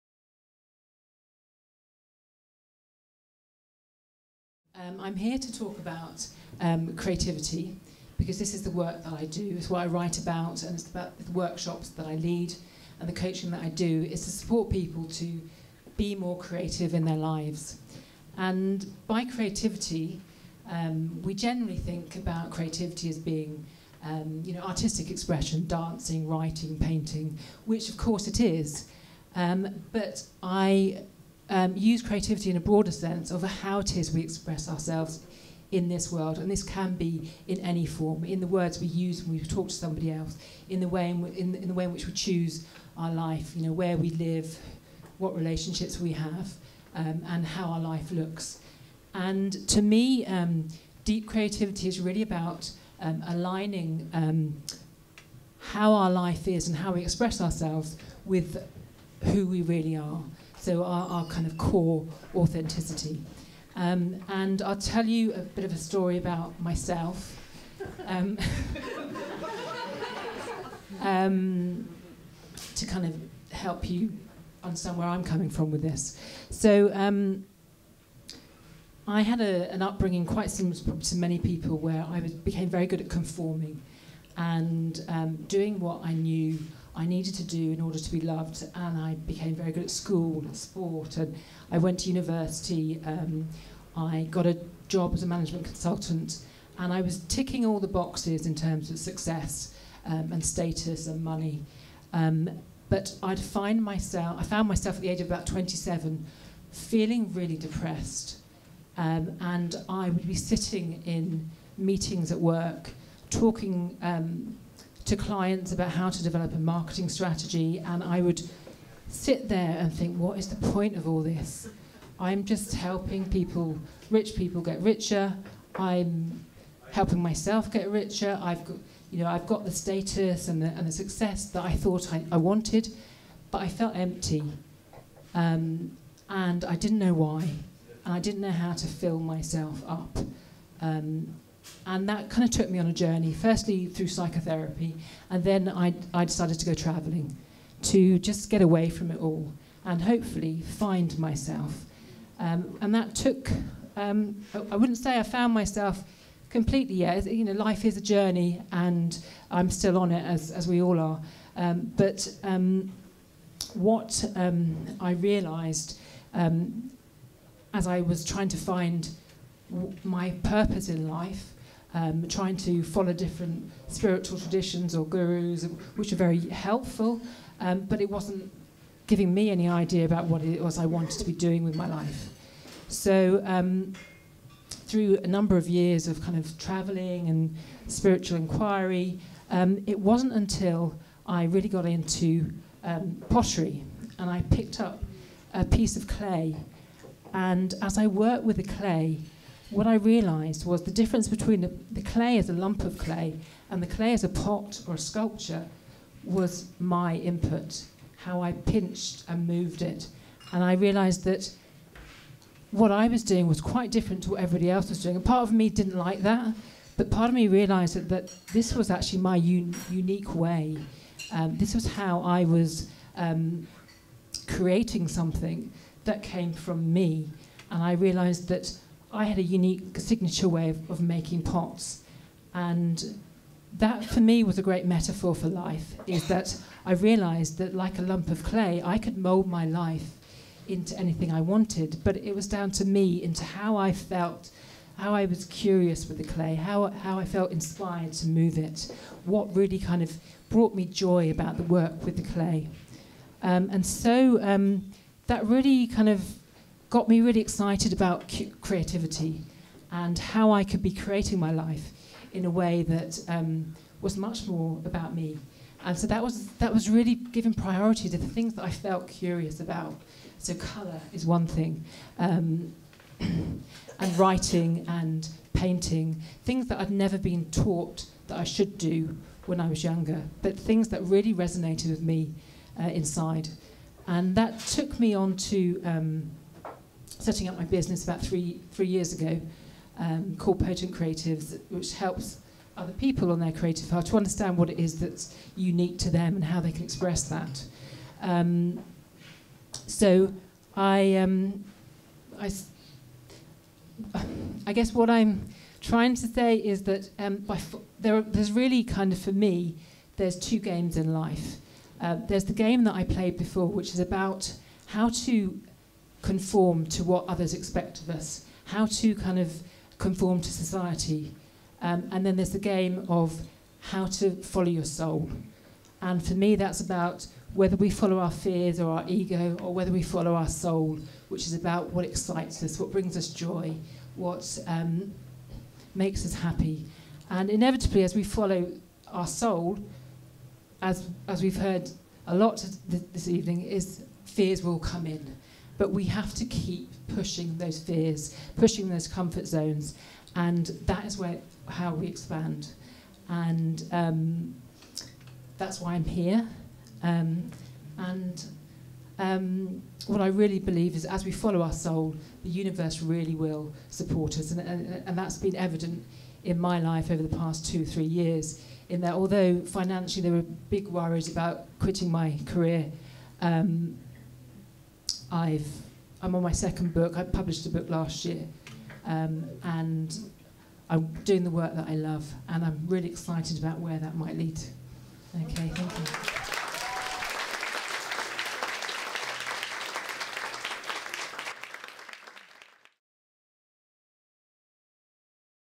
SARAH SAMUEL: “Deep Creativity"
Drawing from a background in mindfulness, business and art, I have developed techniques that connect with the heart of what we want to create in the world, that catalyse creative flow and that bring visions into reality. I take participants on a journey into awareness of themselves so that they can gain inspiration, face resistance and move forward with confidence.  I am published author of “Mindful Crafting: A Maker's Creative Journey" released by Leaping Hare Press and I am currently writing my second book entitled Mindful Travel: Exploring the World, Discovering Yourself”.

Want to come to the next event?  Search Inspiring Talks Brighton in Eventbrite to get tickets.